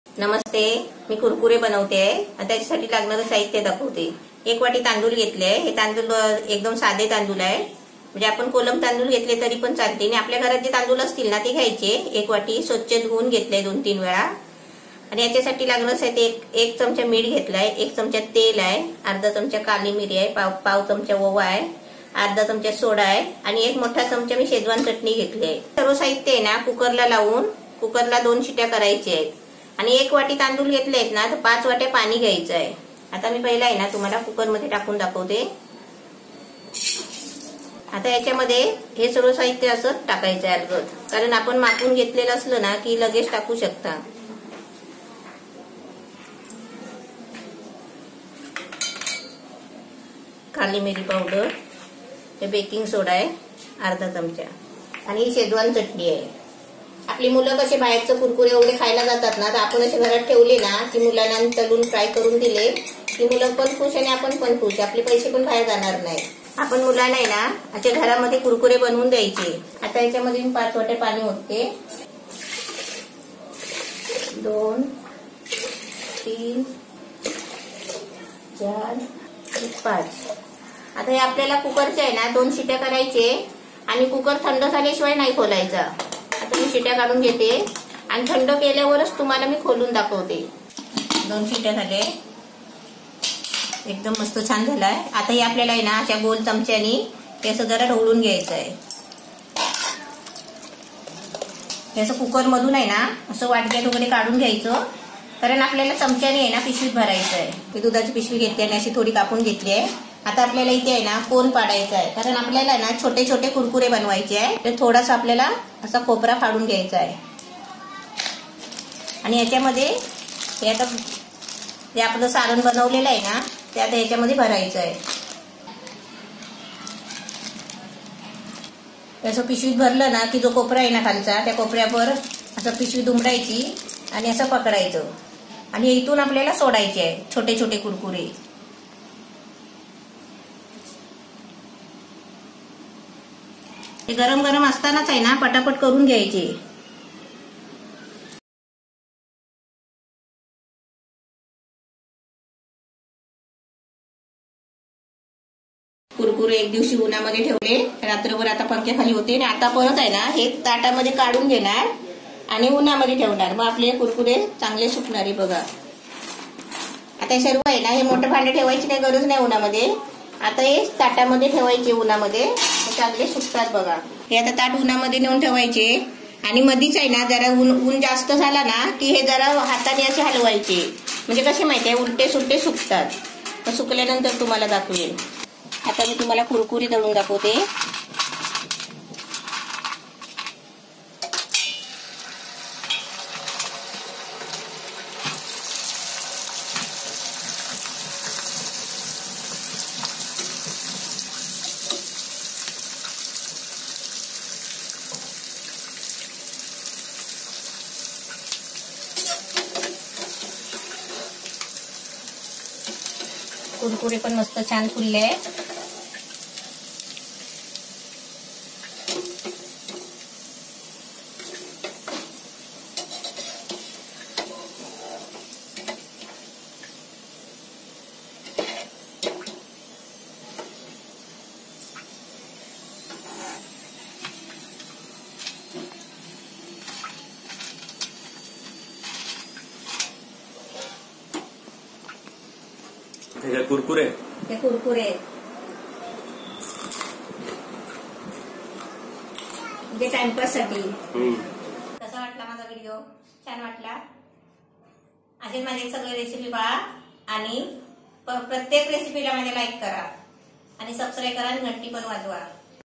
कुरकुरे बनाने की विधि | How to Make Kurkure at Home | Homemade Kurkure Recipe With English Subtitles
Kurkure Ingredients

1 Medium bowl Uncooked Rice
(Rice is Cleaned with water)
1 tbsp Salt
1 tbsp Oil
1/2 tbsp Black pepper
1/4 tbsp Carom Seeds(Ajwain )
1/2 tbsp baking Soda
1 tbsp Schzewan Chutney

7) Potato Kis : Will be uploaded
8) Potato Wafers : Will be uploaded
9) Gavariche Sandge : Will be uploaded